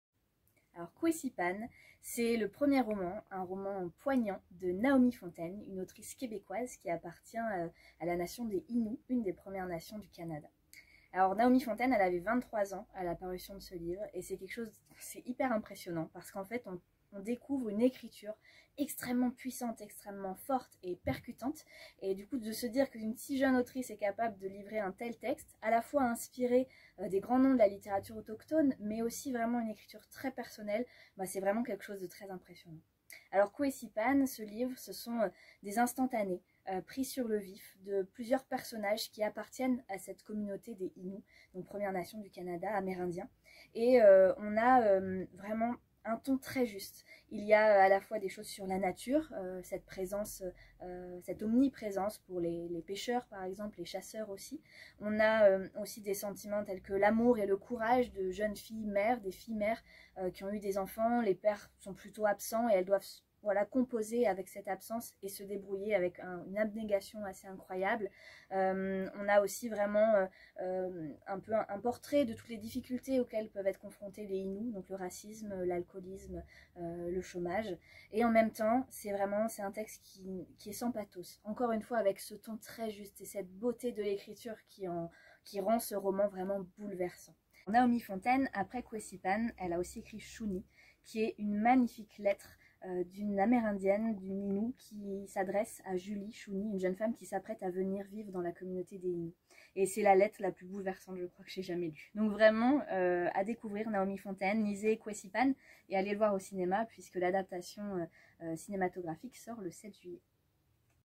"Kuessipan" de Naomi Fontaine par Marianne
Le 15 juillet 2021, à 20h30, nous aurons le plaisir de tenir un stand au cinéma Le Méliès Lille métropole, pour la projection du film "Kuessipan", libre adaptation du roman de Naomi Fontaine, publié aux éditions Mémoire d'encrier et que nous avions adoré !
En attendant la sortie du film, et pour vous mettre l'eau à la bouche, Marianne vous présente le roman dans une petite vidéo !